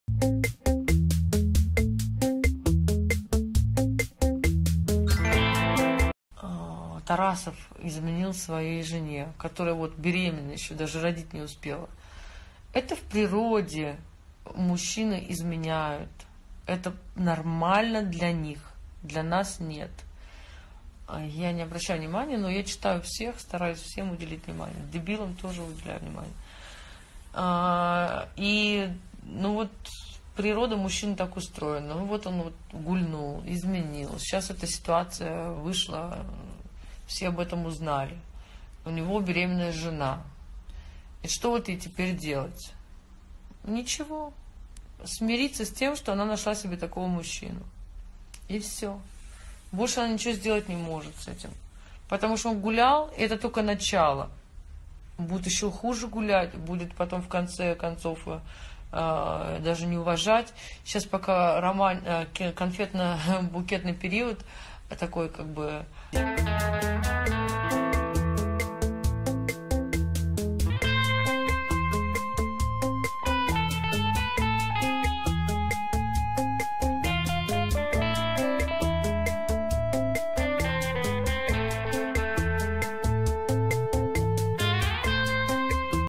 БОНЯ ПРО ИЗМЕНУ ТАРАСОВА КОСТЕНКО (ondom2.com)
В этом выпуске новости и слухи  про Викторию Боню, Дмитрия Тарасова и Настю Костенко.